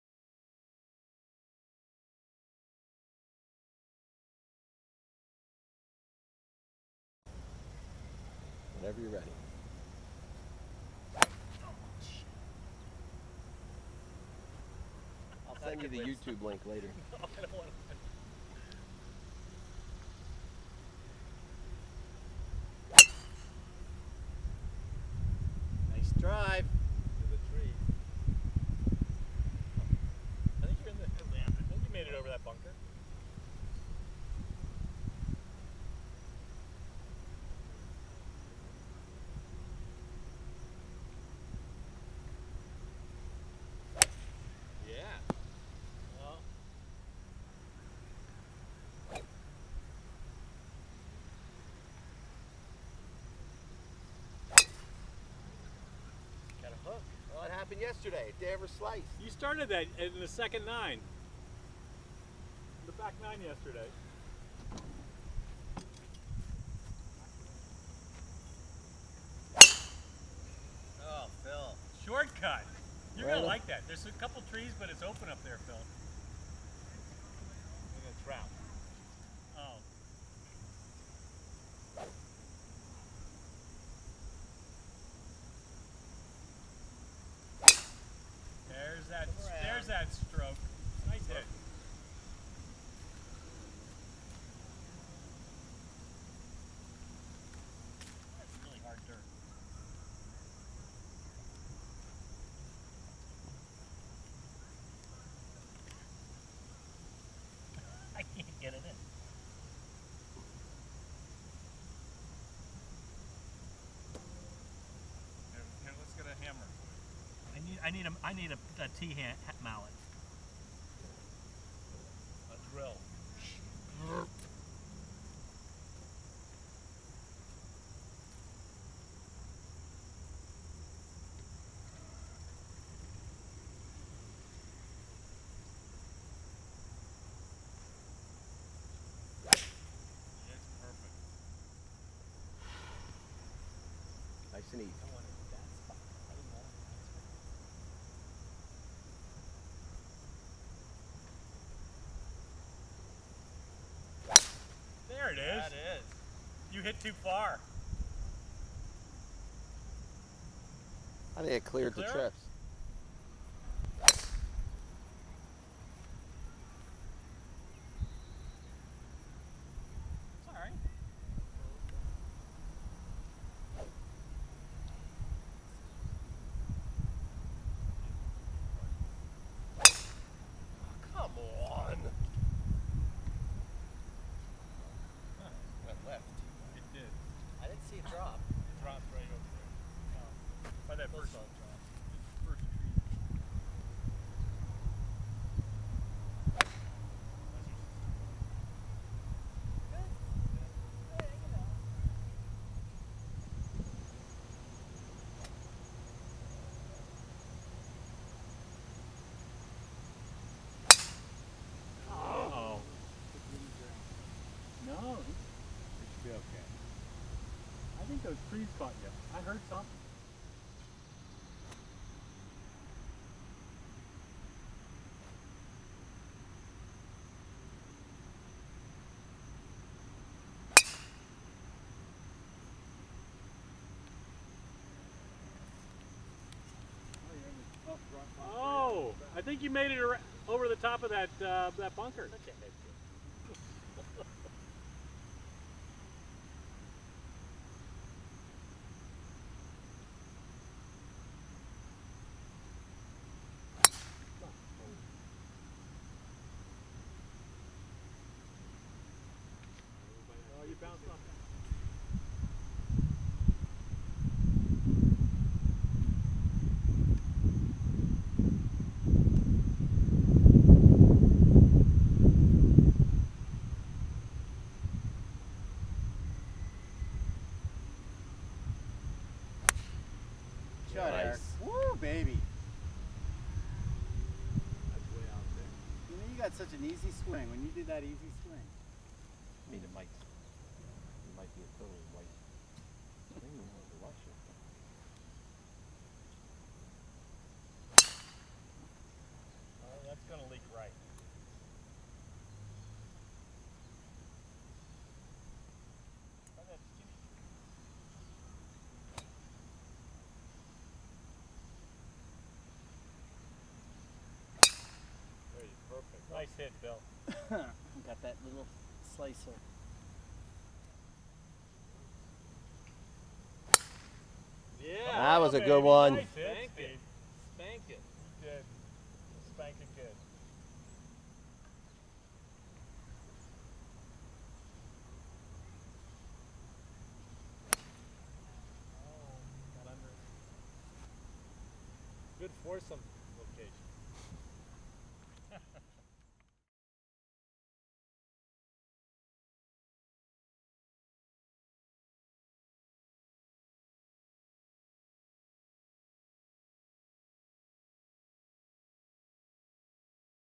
Bosin Open 2014
Bill, Steve, Eric and Phil compete for the coveted Bosin Open Jacket in the 2014 Bosin Open held at Downingtown Country Club.